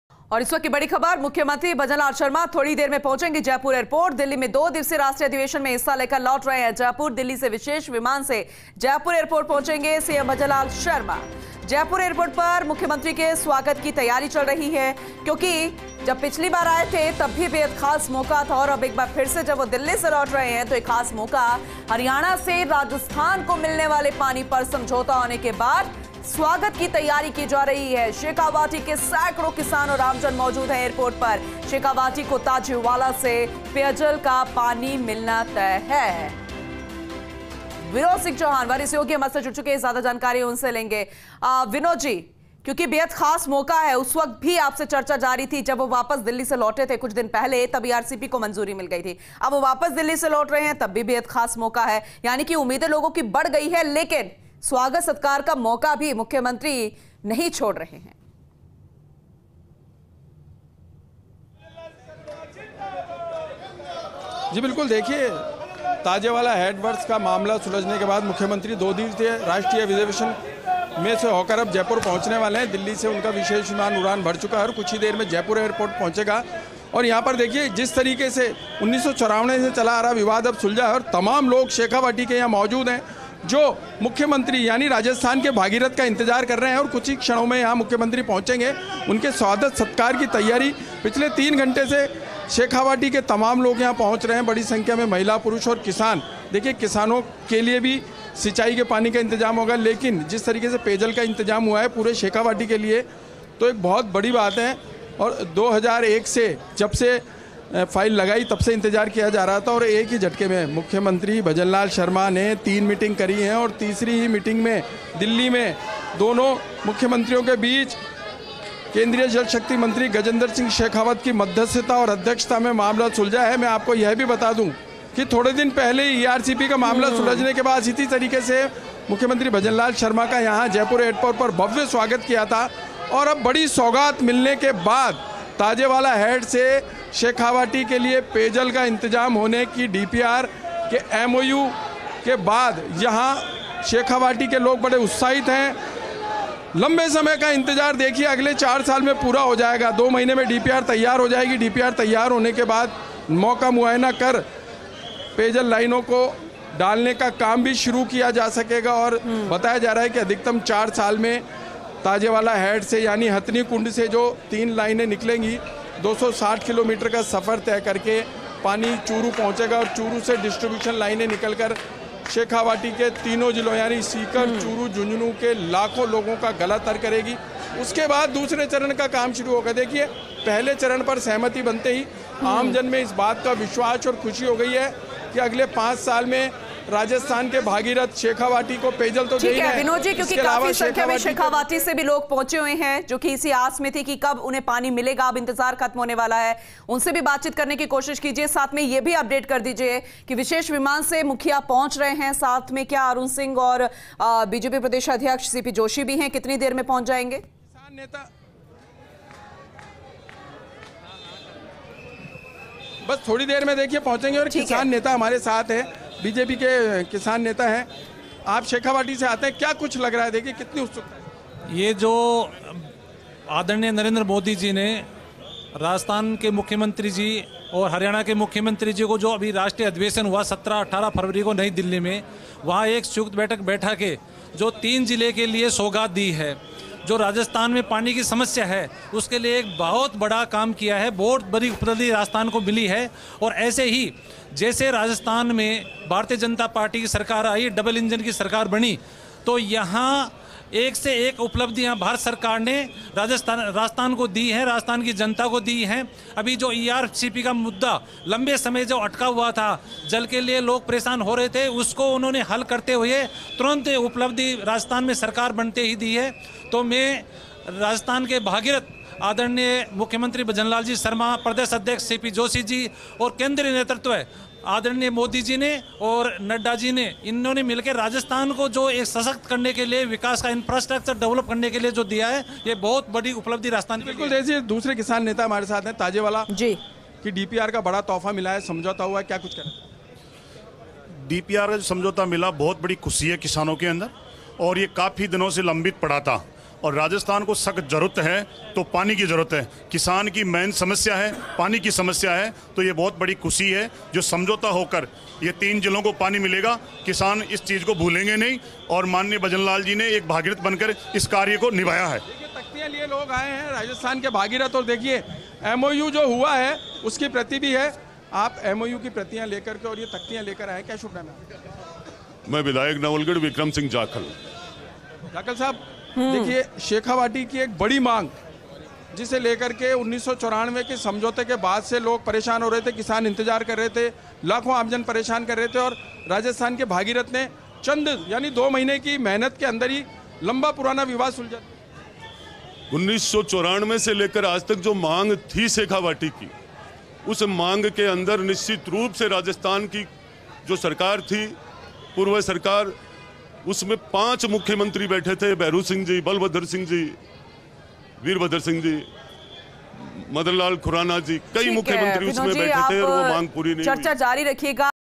CM Bhajanlal Sharma के स्वागत की तैयारी, Jaipur Airport पर जुटे दिग्गज नेता
About this Video:
CM Bhajanlal Sharma के स्वागत की तैयारी, Jaipur Airport पर जुटे दिग्गज नेता | 19 February 2024